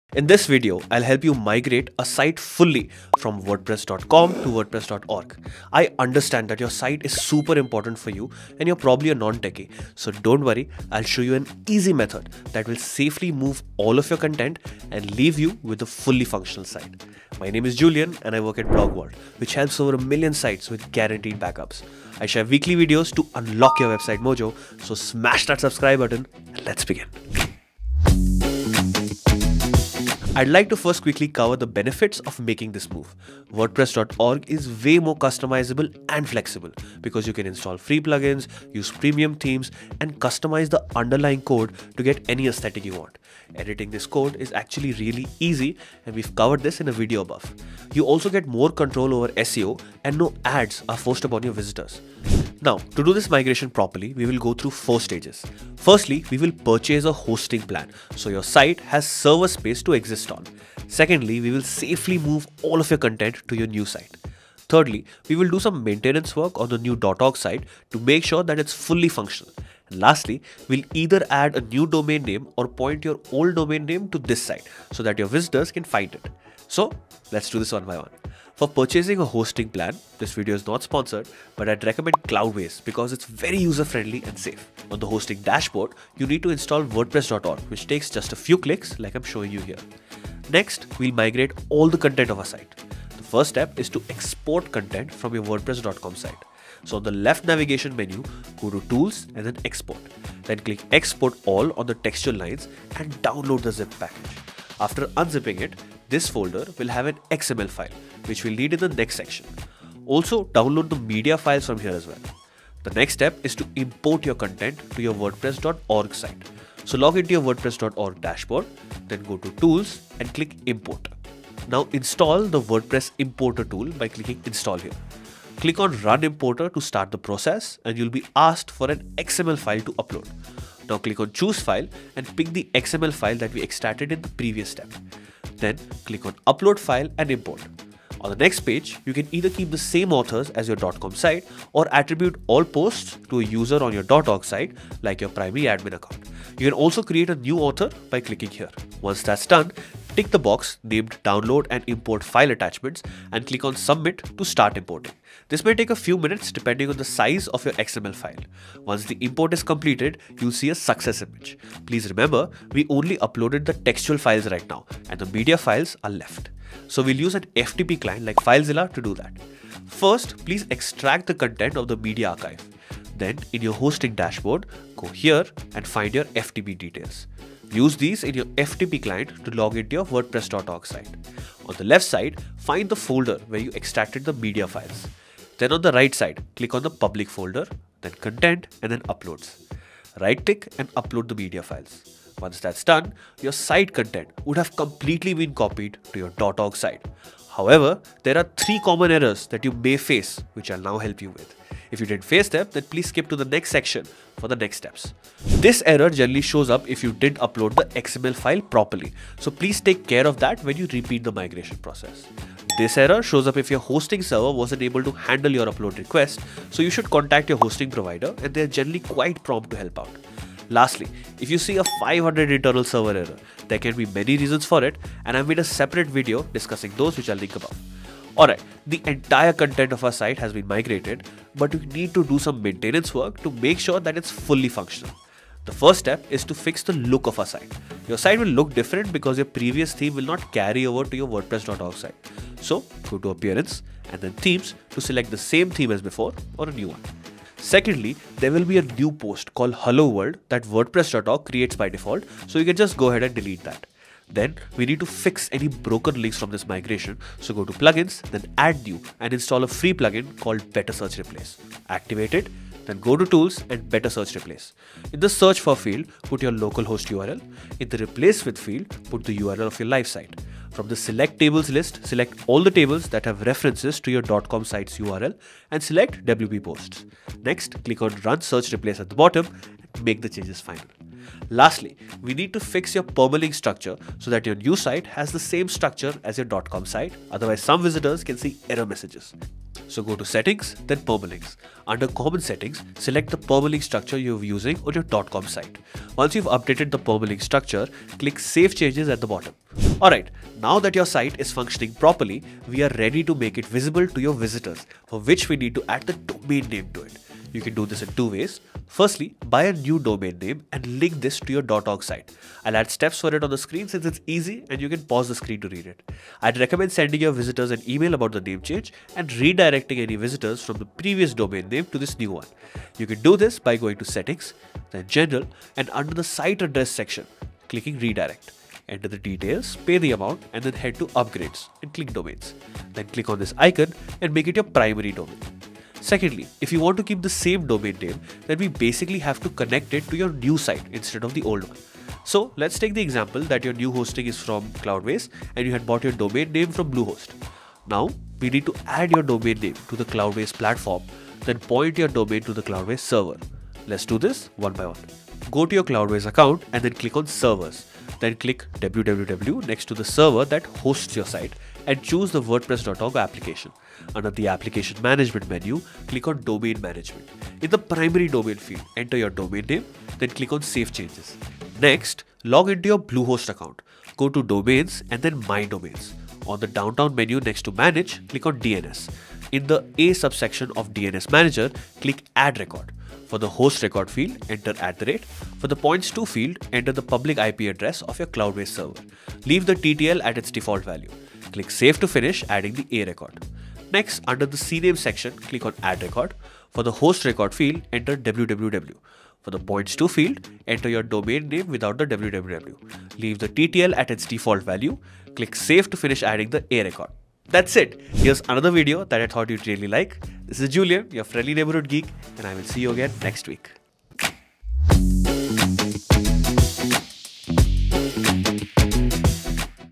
How to move from WordPress.com to WordPress.org (Free Migration)
I'll show you a safe, easy & quick method to move your entire site from WordPress.Com to WordPress.Com. DONT FORGET - your new WordPress.Org site will need bulletproof backups & complete security

00:00 - Need to Know!
00:31 - Why move from WP.com to WP.org
01:20 - 1. Purchase Hosting & Install WordPress
01:32 - 2. Migrate all Content
03:16 - 3. Common Errors
03:49 - 4. Activate WP.org site
05:00 - 5. Publish WP.org site
05:42 - Bonus: Link old domain